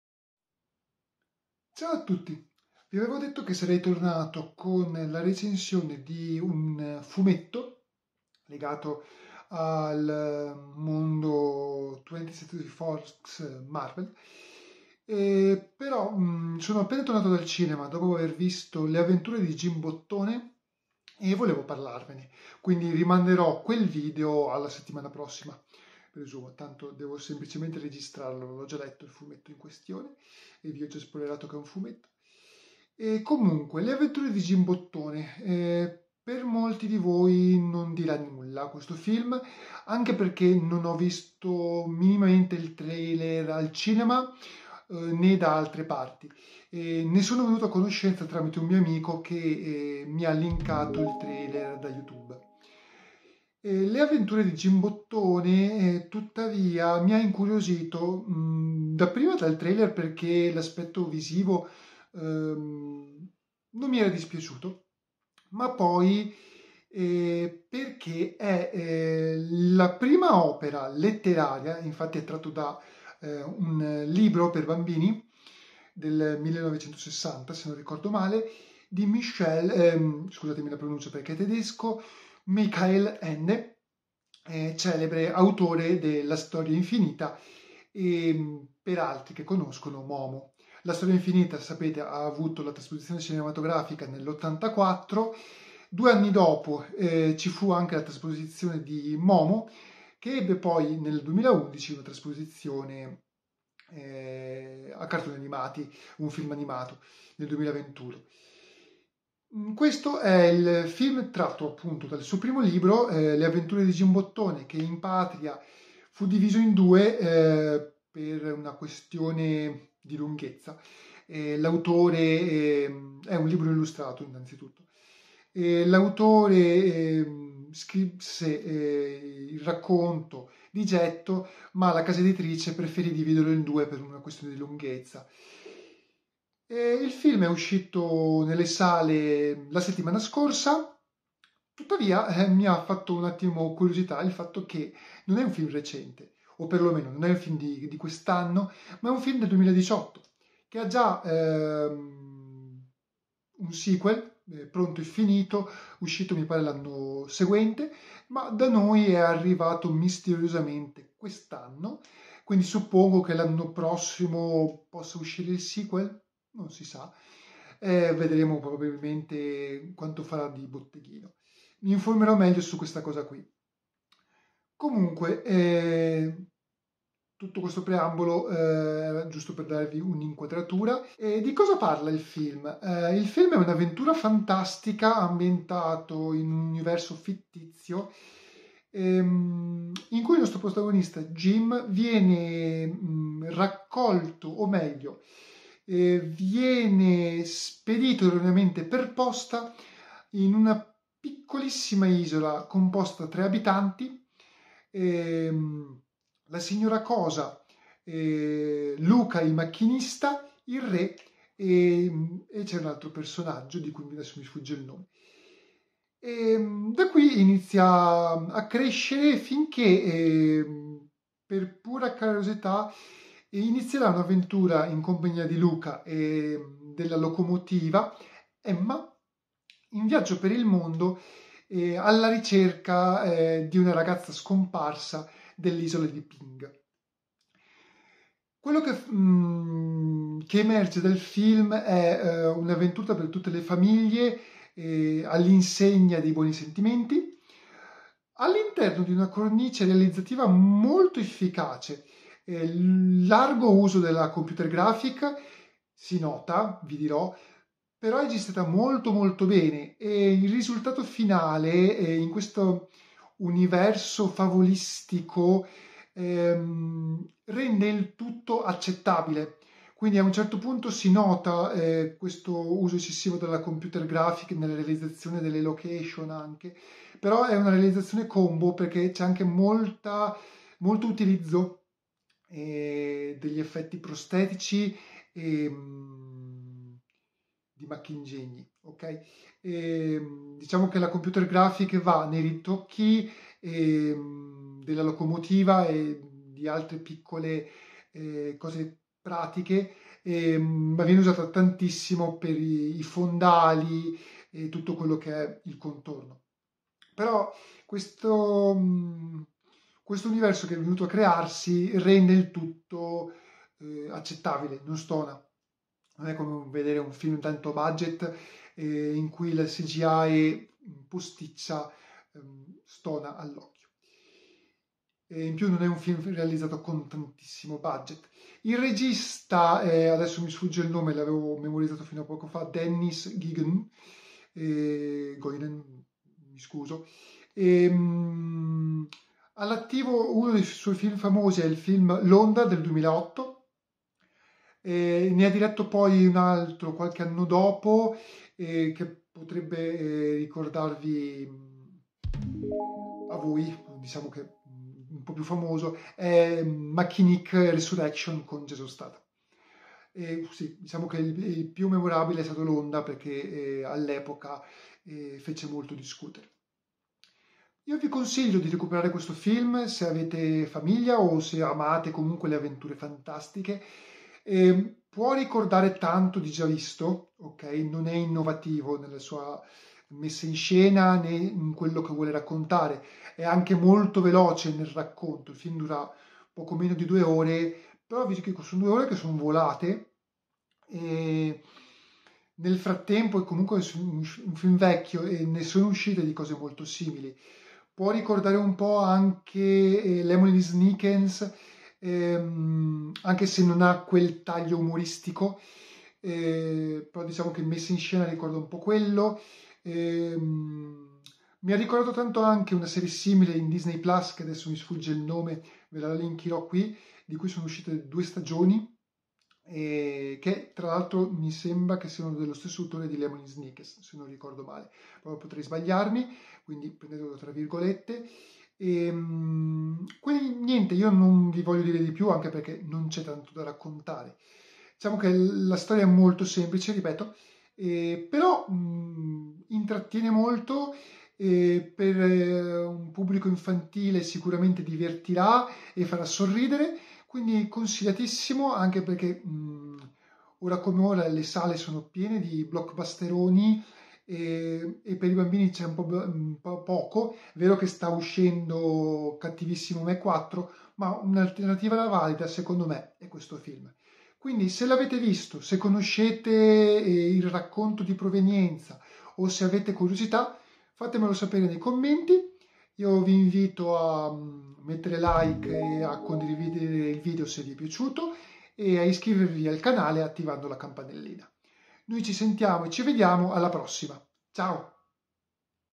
Let's talk about... Le avventure di Jim Bottone.
In questo video espongo la mia opinione sul film del 2018, ma da noi uscito ad agosto 2024, Le avventure di Jim Bottone, diretto da Dennis Gansel e tratto dal libro per bambini di Michael Ende.